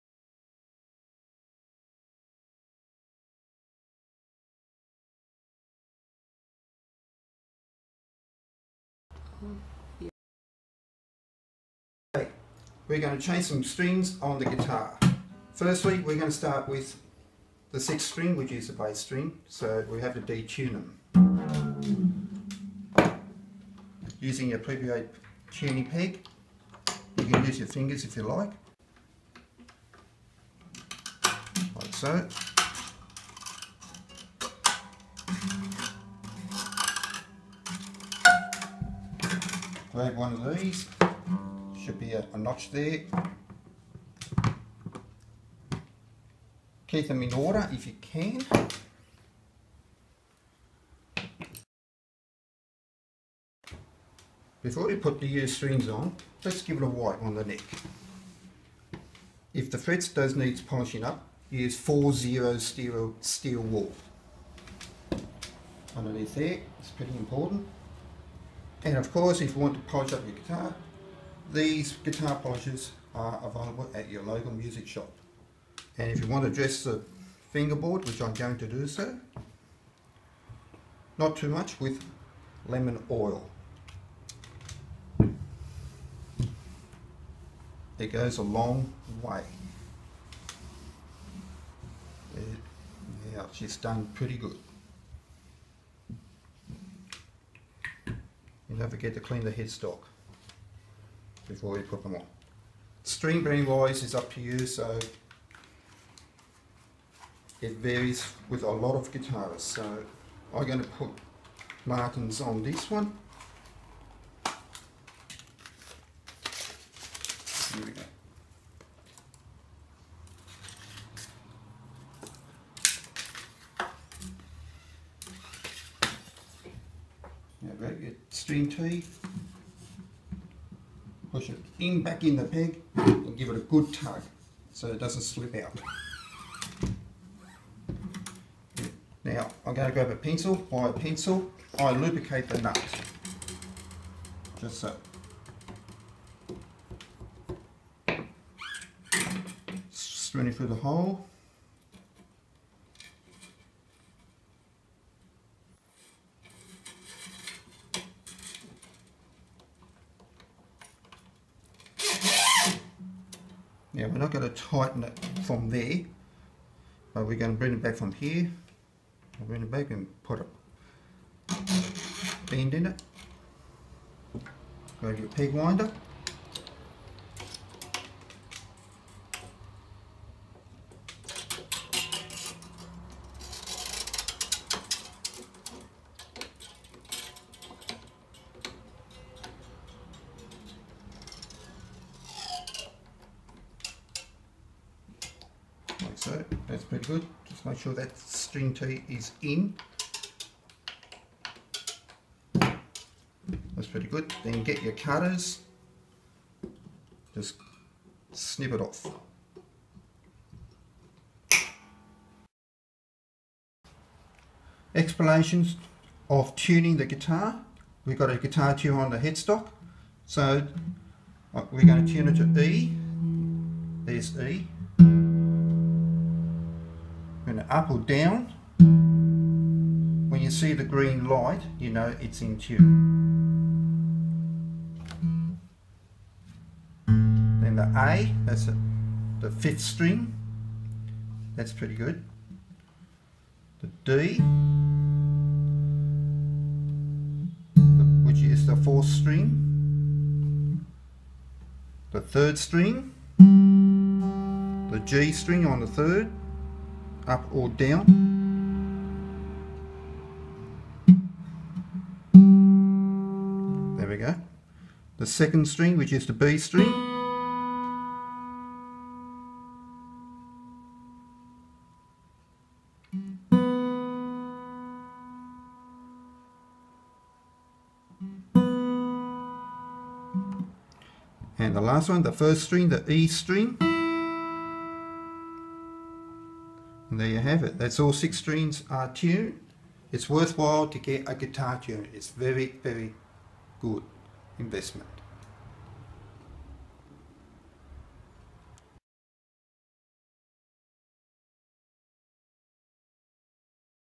Newcastle Steel City CMC Guitar Stringing & Tuning easy Tips 4
Newcastle Steel City Country Music Club is open with a band and walk up artists every Tuesday nights 7pm start at West Wallsend workers club NSW Australia .
Music Tips 4 -David Mason is one of our devoted member who is always at the club setting up and packing up after our nights . David also helps out with the sound while people are singing .David did a great job on guitar stringing and tuning in this clip with easy to follow instructions .

 Facebook -Newcastle Steel City CMC